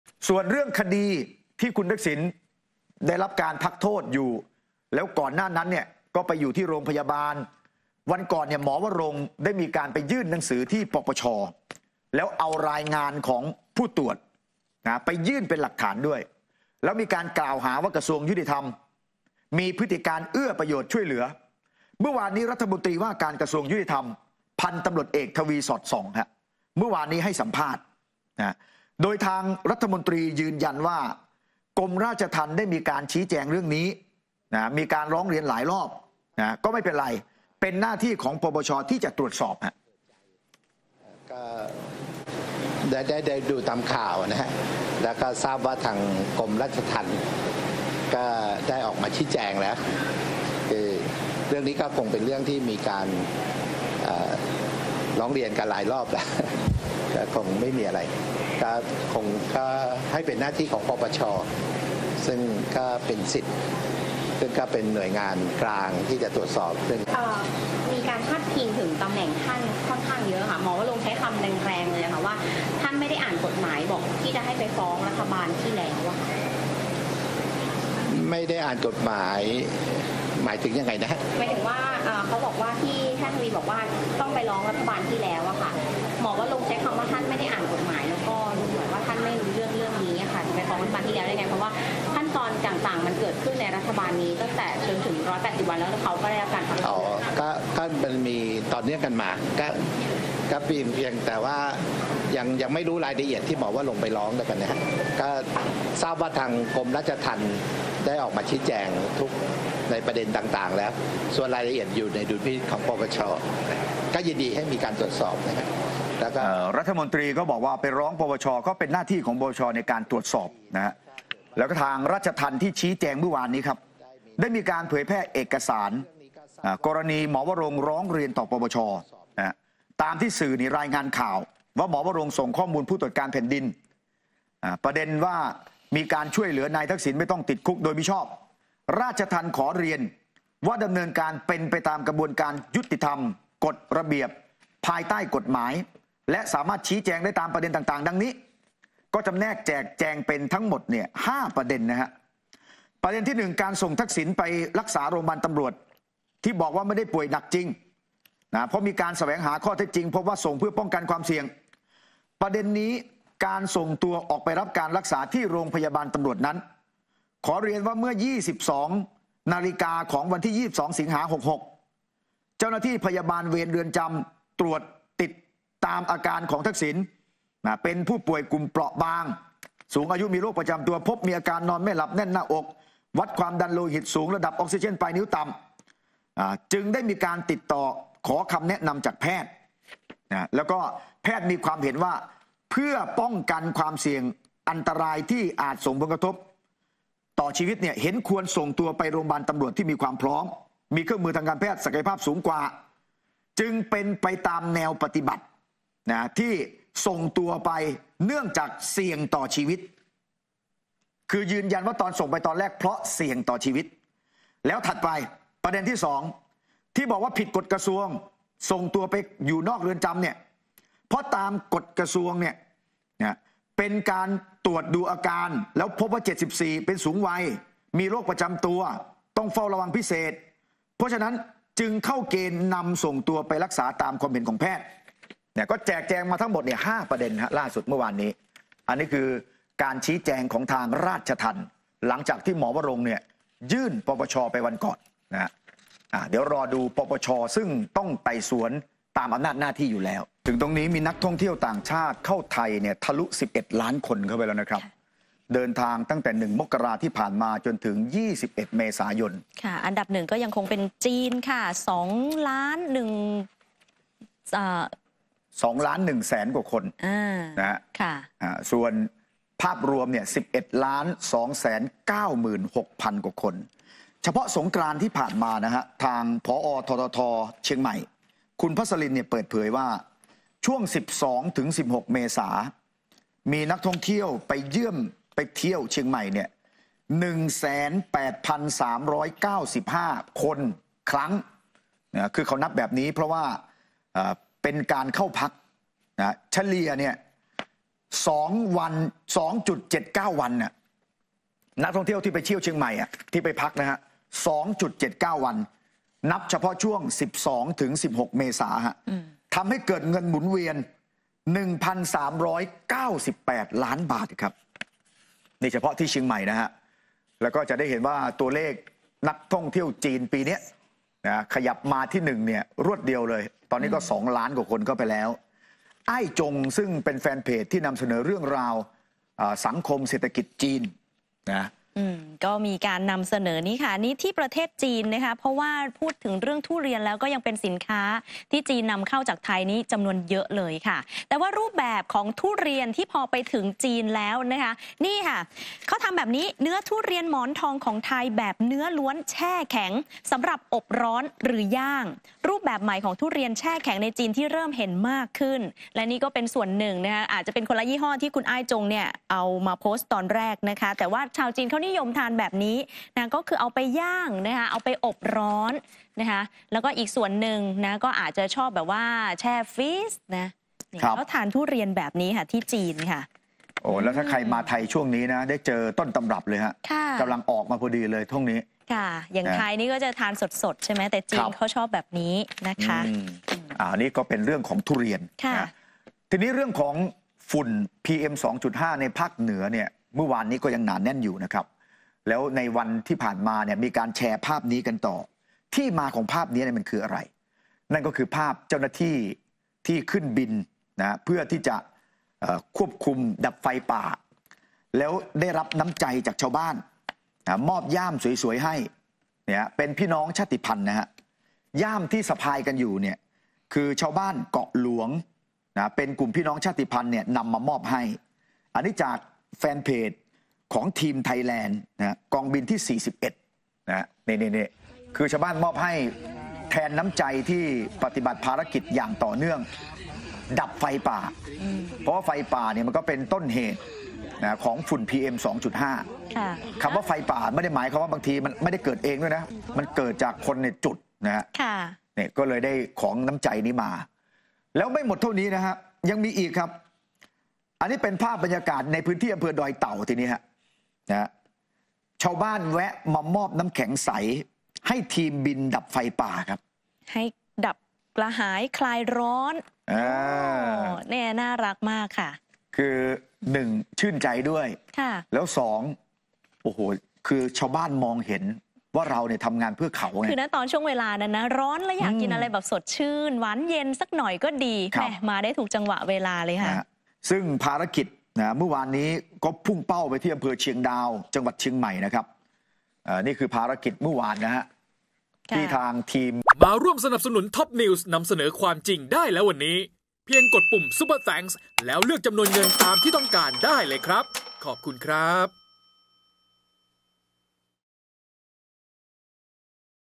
"ทวี" ไม่กังวลพร้อมให้ตรวจสอบ กรณี "หมอวรงค์” ร้อง ป.ป.ช.เอาผิดปมช่วย "ทักษิณ" | TOPNEWSTV
"ทวี" ไม่กังวลพร้อมให้ตรวจสอบ กรณี "หมอวรงค์” ร้อง ป.ป.ช.เอาผิดปมช่วย "ทักษิณ" ป่วยทิพย์